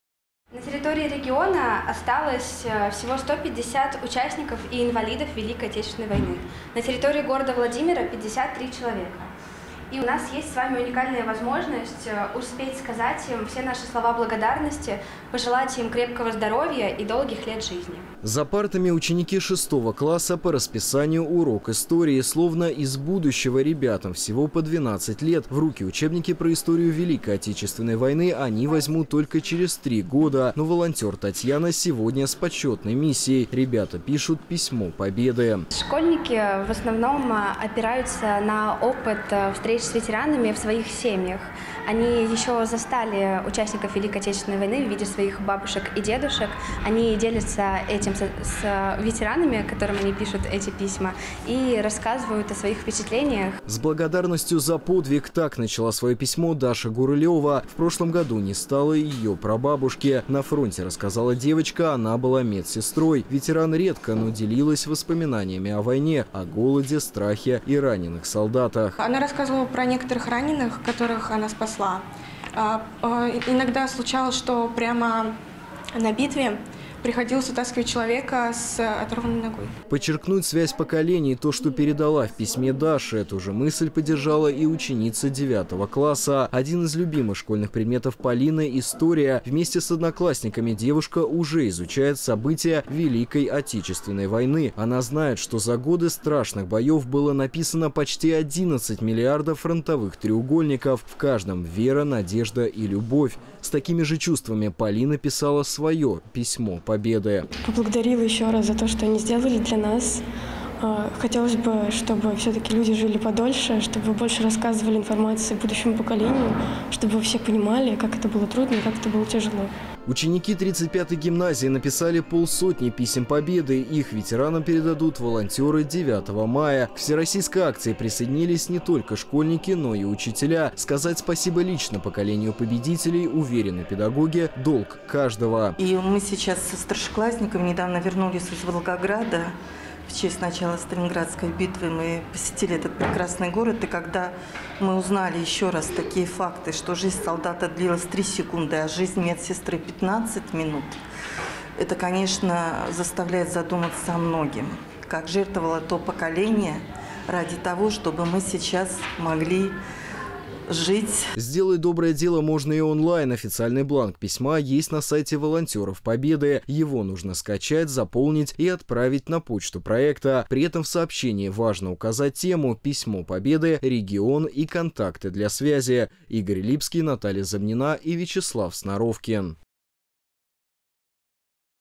Во Владимире проходит всероссийская акция «Письмо Победы» (2022 05 06)
Школьники поздравили ветеранов Великой Отечественной войны с наступающим праздником и специально для них написали «Письмо Победы». Так называется всероссийская патриотическая акция, к которой присоединились ученики 35-й гимназии из Владимира.